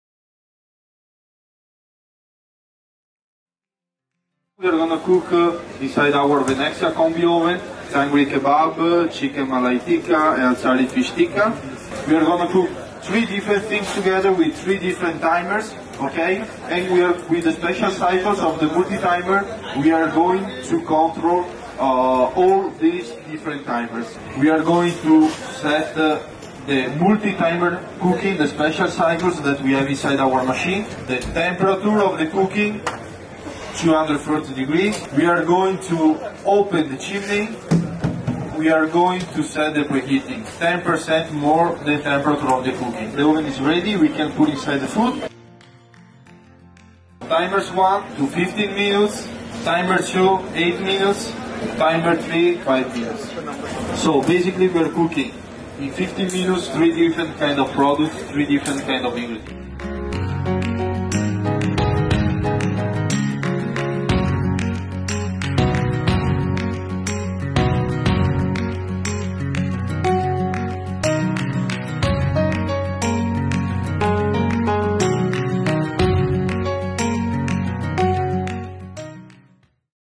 Cooking with Venexia Combi oven at AAHAR FAIR 2023
VENIX Corporate Chef @Matteo Righetto showed the potential of the Venexia Combi oven by cooking Tangdi Kabab, Chicken Malai Tikka & Achari Fish Tikka at the 37th edition of AAHAR - The International Food & Hospitality Fair which took place at Pragati Maidan, New Delhi, India from 14th to 18th of March, 2023.